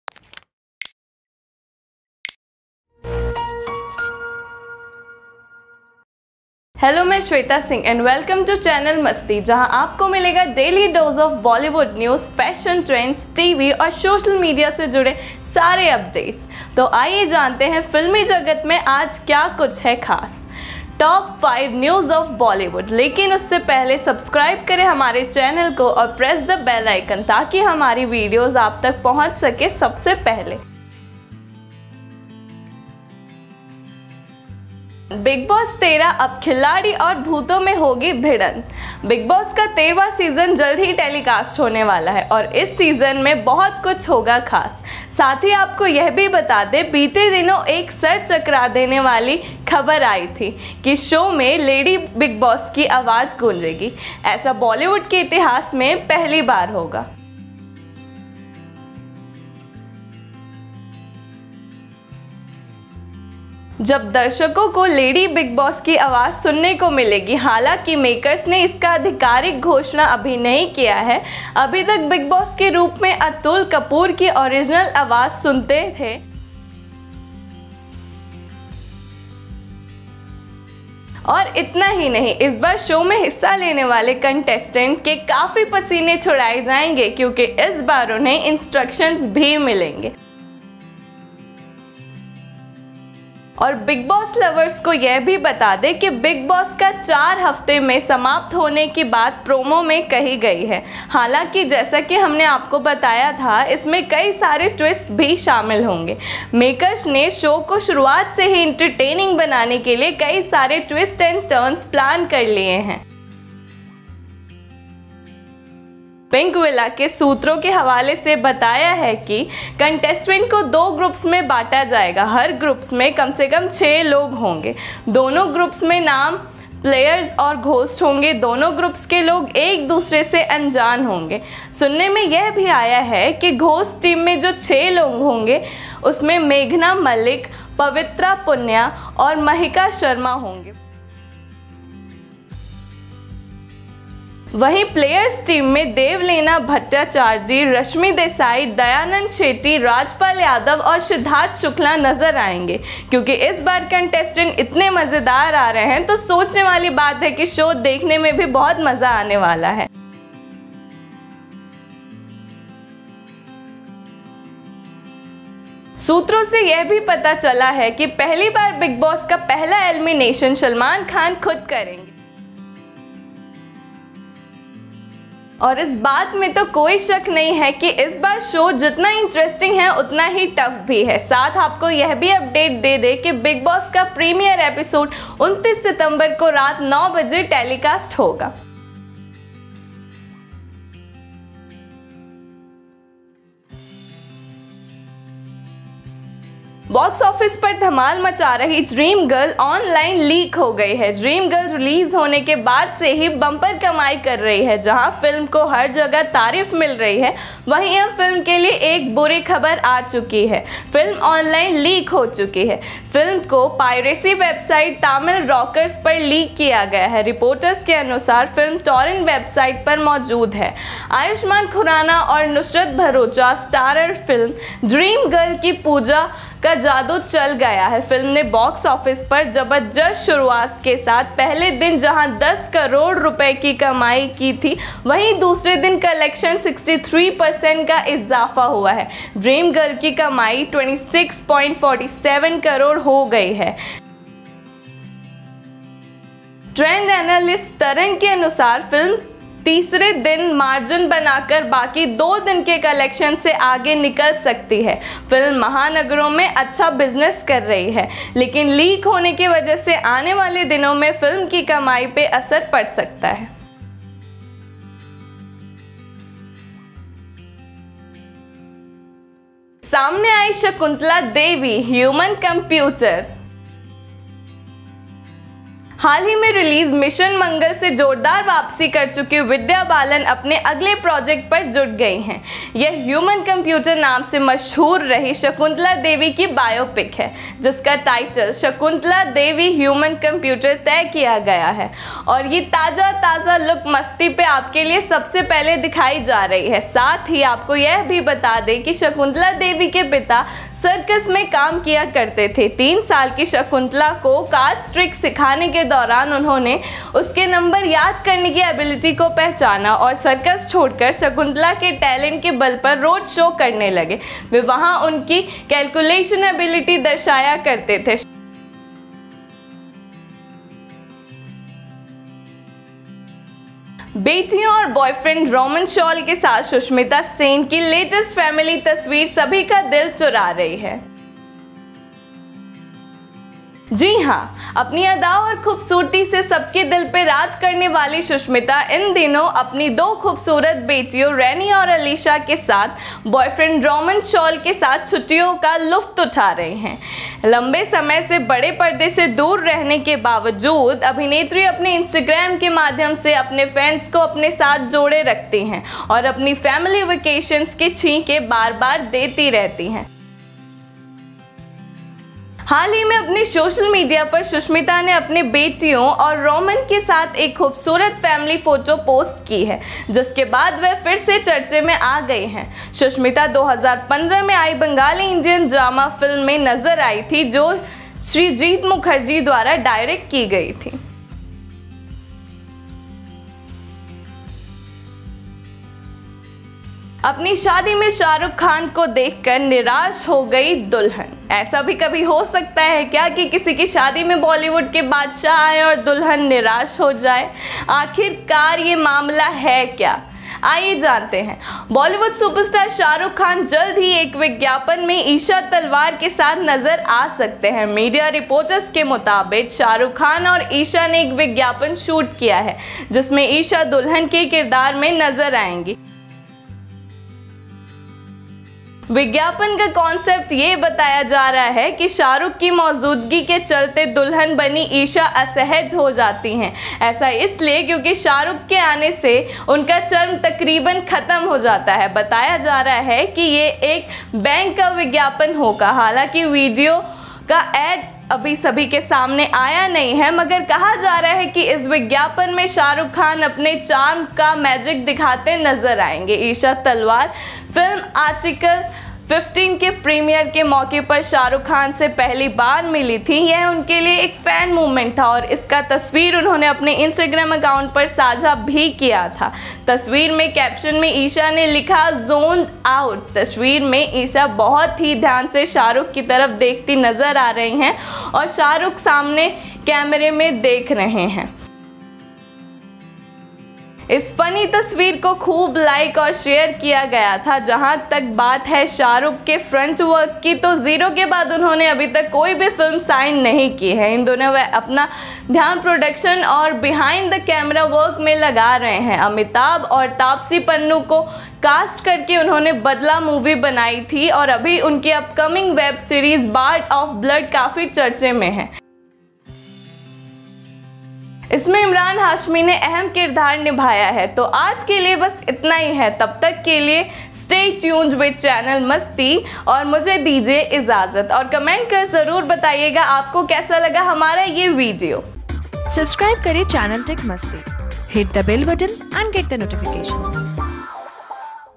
Top 5 Bollywood News : Big Boss season 13 क्या है नया क्या है खास जानिए | Salman Khan
CHANNEL TEC MASTI EXCLUSIVE BOLLYWOOD VIDEOS